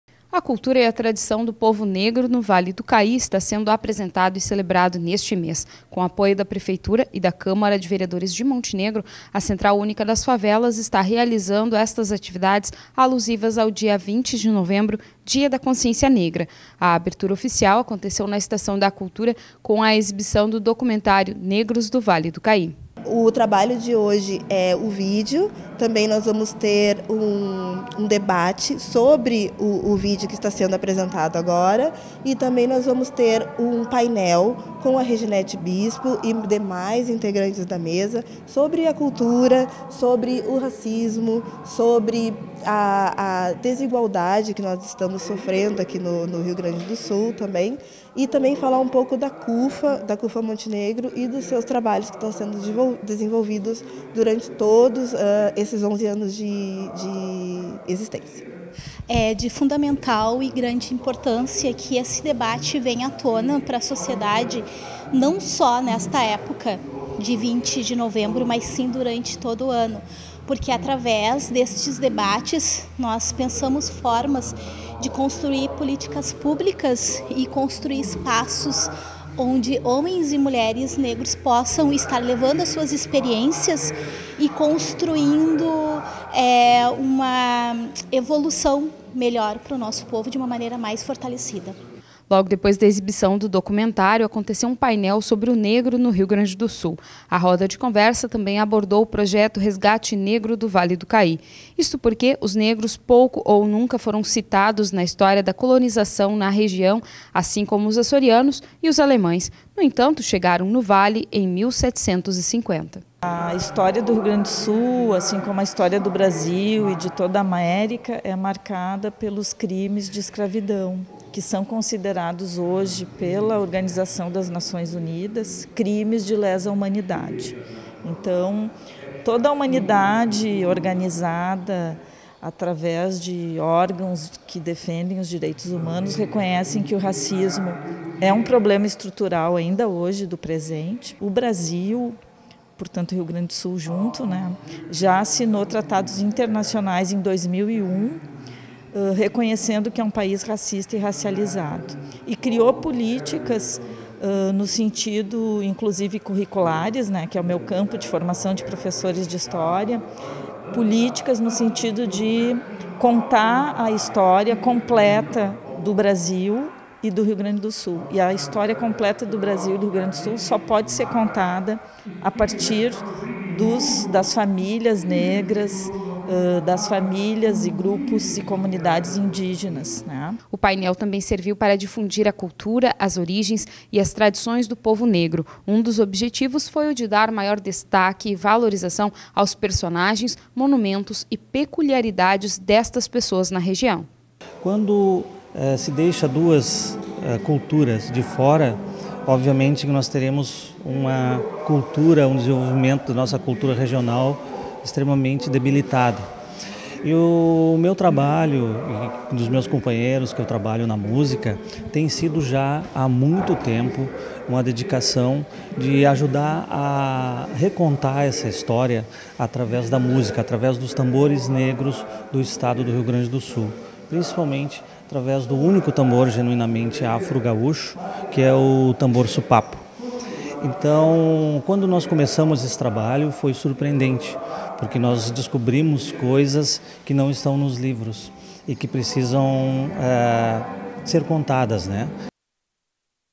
Cultura e Tradição são temas das atividades do mês da Consciência Negra.
A Central Única das Favelas está promovendo uma série de atividades alusivas ao mês da Consciência Negra. Um documentário sobre o negro no Vale do Caí foi exibido dentro dessa programação especial.